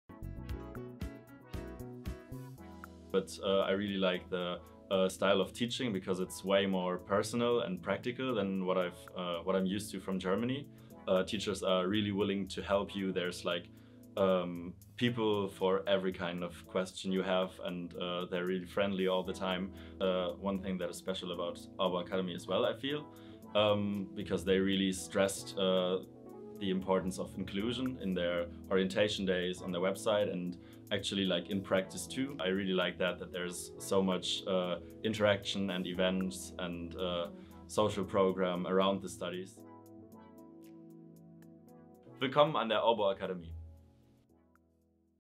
Yannick Zapfe - Student of the Master's Degree Programme in Information Technology
Yannick Zapfe gives his view on what it's like to study in Finland and at the Master's Degree Programme in Information Technology